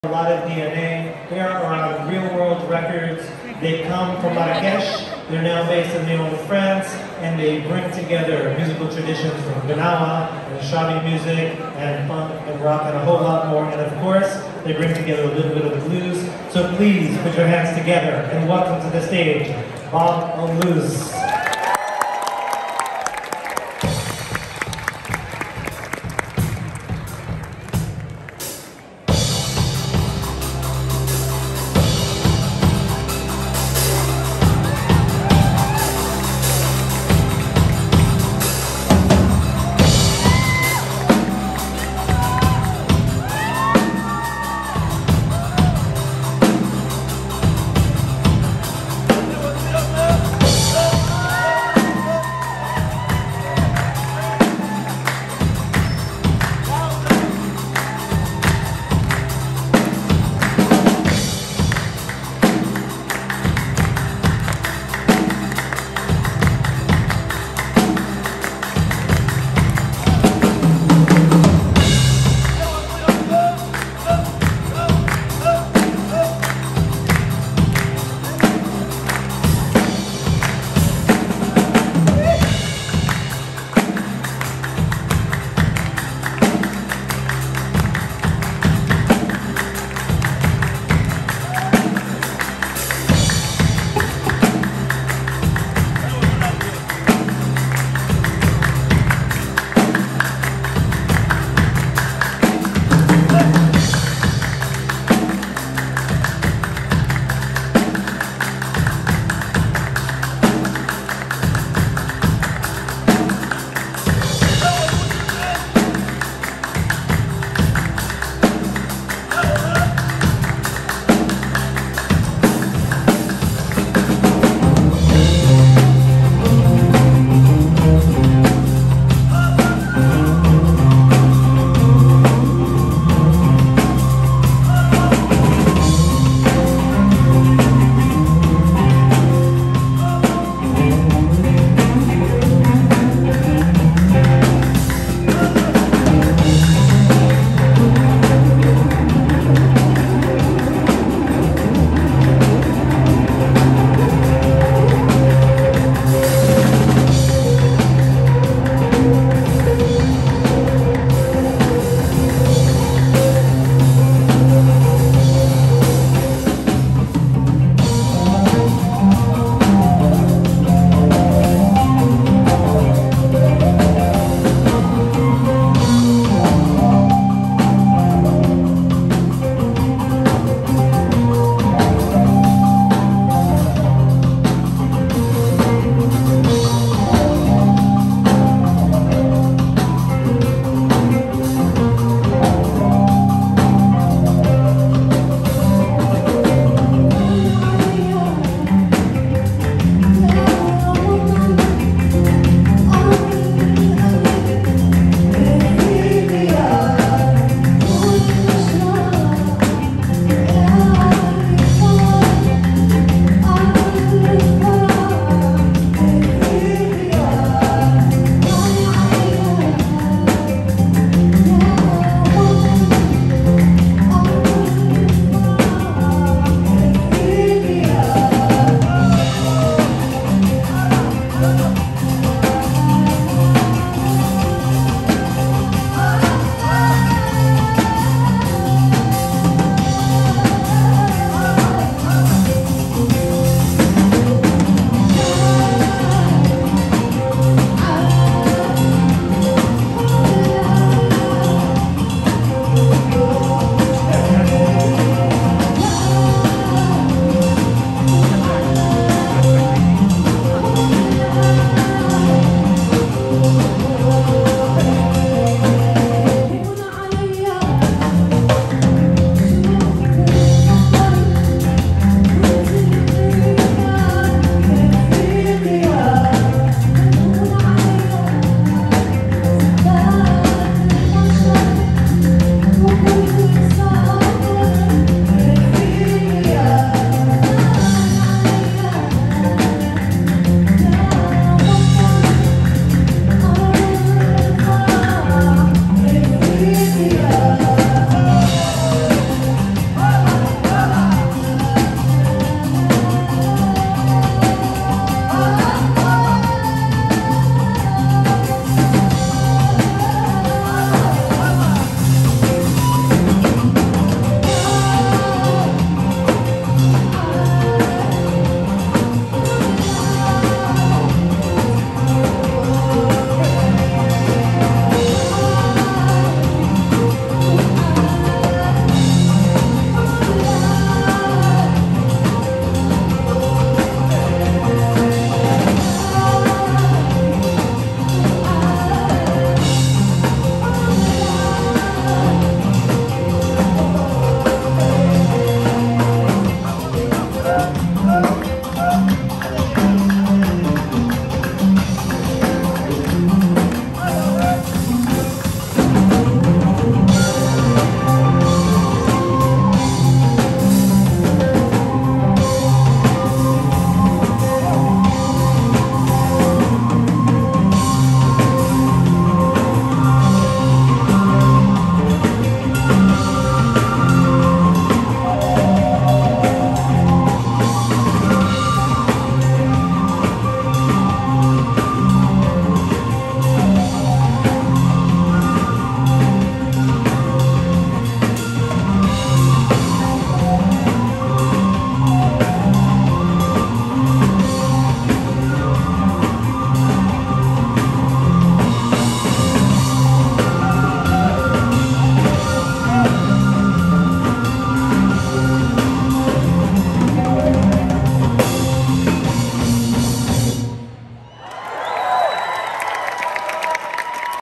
Bab L’ Bluz perform at Global Fest, Leon & Norma Hess Grand Promenade, Lincoln Center, Jan.14, 2023
You’ve happened upon Bab L’ Bluz, a Moroccan-French power quartet that is reclaiming the blues for North Africa. Created in 2018 in tribute to Gnawa culture, the centuries old Moroccan practice rooted in Islamic and sub-Saharan African traditions and music played with the guembri, the three-stringed bass-lute, Bab L’ Bluz has commanded attention, opened doors — ‘bab’ means ‘gate’ in Arabic — from the off.

“The African blues includes other pentatonic music: Mauritanian hassani. The Berber music found in Morocco’s Atlas Mountains. The music of Mali, which is the true source of the blues as well as the true source of Gnawa music.”

Yousra and Brice co-founded Bab L’ Bluz while honing their skills on the guembri, exploring the possibilities of the iconic instrument as they composed in their studio in Marrakech. The resultant outpouring of creativity is showcased across the album’s ten tracks.

Yousra grew up in El Jadida, a fortified town on Morocco’s windswept Atlantic coast, playing guitar and performing songs by Lebanese diva Fairouz (their voices have the same melismatic quaver). Listening to Janis Joplin, Oumou Sangaré and Erykah Badu. Attending the annual sprawling Gnawa Festival (the so-called ‘largest jam session on the planet’) in nearby Essaouira.

Written mostly in Moroccan darija, Yousra’s lyrics are imbued with light and shade, warmth and nuance. They are, out of necessity, ambiguous: “I write between the lines about current subjects and society’s problems. The meaning can be understood.”